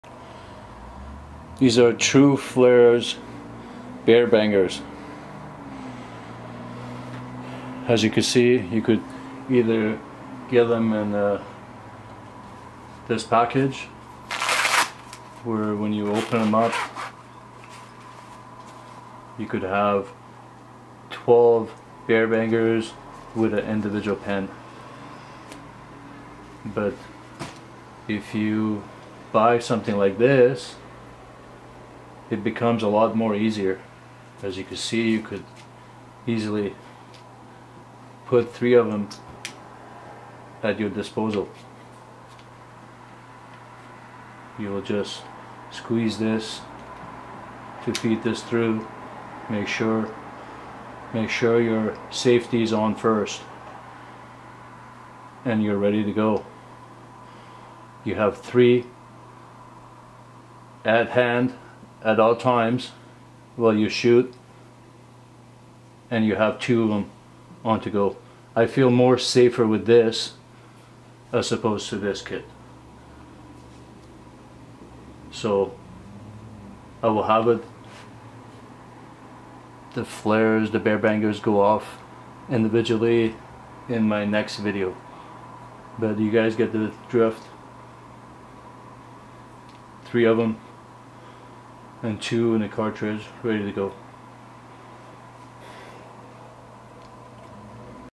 Triple Tru Flare Pen Launcher for Bear Bangers
Tru. Flare pen launcher along with Tru. Flare Bear Banger;  Quick video on how to fire a bear banger,  Something I will have with me this summer as I plan on photographing more wildlife.

Why load one Bear Banger or Flare when you can have three ready to go on a moments notice.  Just spin the top to launch the next Bear Banger or Flare.

The triple launcher has the ability to deter animals from a distance and provide effective signaling with various flares. It's small profile and compact cartridges easily slip into a pocket or bag for any wilderness occasion.

Outfitting the Pen Launcher with a Bear Banger or Whistle cartridge provides effective animal scarring while keeping you safe from a distance. Commonly used on bears, cougars, coyotes, birds, elk and other animals.

Need to get someones attention? The flare cartridges are visible for up to 3 km during the day and an impressive 12km at night.

If you are going to pack something for the wilderness it's valuable to have multiple purposes. The Pen Launcher is a tool with a variety of attachments and a compact design. The Thumb Level Model works by pre-cocking the firing pin into a safety notch allowing for simple one handed operation.

Specifications:

Works with 15mm center fire cartridges only
Durable metal design
Pull style activated through a spring loaded rod
Thumb style trigger for one handed firing
Light weight: 50 g
Compact size: Length- 14 cm (5.5") Diameter- 1.2" (3.0 cm) to 3.8 cm (1.5")

Mine is actually the older model:

•that has the extra black barrel so I can load 3 Bear Bangers into it,

•The newer version doesn’t have this Black Barrel & extra attachment as they have stopped making these attachments to make this unite smaller to fit inside your pockets better by simply drilling into the orange ring which now you have to screw on each Bear Bangers onto that piece & once your done firing all three that you’ll need to unscrew all three empty caps(How I wish they still had my Black Barrel extra Attachment cause once I’m done firing all three Bear Bangers, all I have to do is to take them out individually & toss them into the garbage without unscrewing anything: